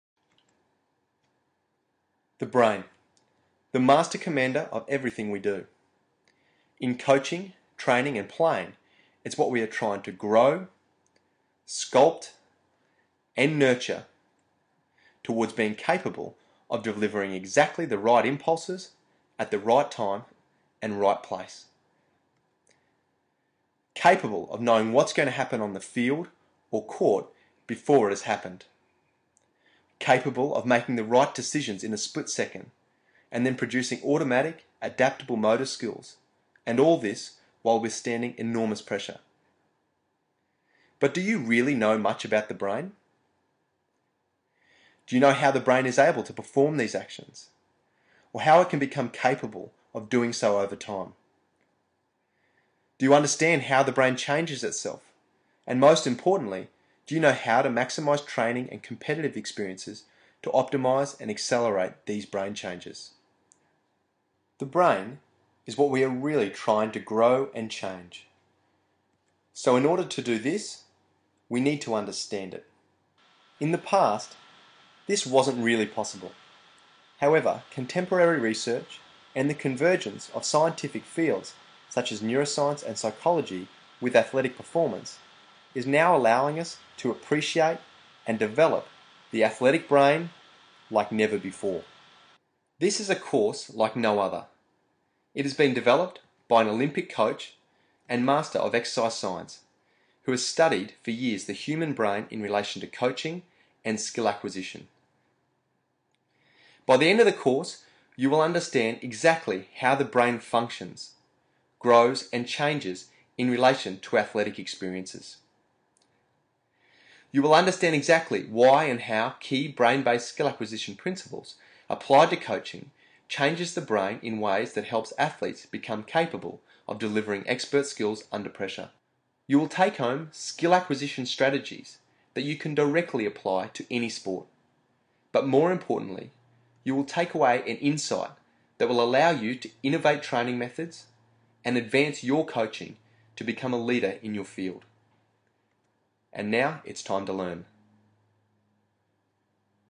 Athletic Brain Training - Neuroscience, Sports Expertise Development, Skill Acquisition & Psychology
Athletic Brain Training Course That Relates Research From Neuroscience, Sports Expertise Development, Skill Acquisition and Psychology
Training the Athletic Brain
Unique brain training course that relates research from brain science and skill acquisition with practical outcomes.

The Brain Training: Training the Athletic Brain Course gives coaches greater insight into how expert sporting skills actually develop over time.  It brings together contemporary research from neuroscience, sports expertise development, skill acquisition and psychology.  In particular, it looks at how to coach most effectively at what ages based on how an athlete’s brain is actually developing. The course is guaranteed to challenge any coach and make an impact on all aspects of a coaching program.

The Brain Training course is structured so that students get grounding in fundamental knowledge in the beginning and delivers practical coaching outcomes in the latter half of the course.

The course is delivered mostly through video lectures along some written lectures and downloadable PDF's.  In total the course will take about 10 hours to complete.

Take this Brain Training course now and discover the brain science of athletics.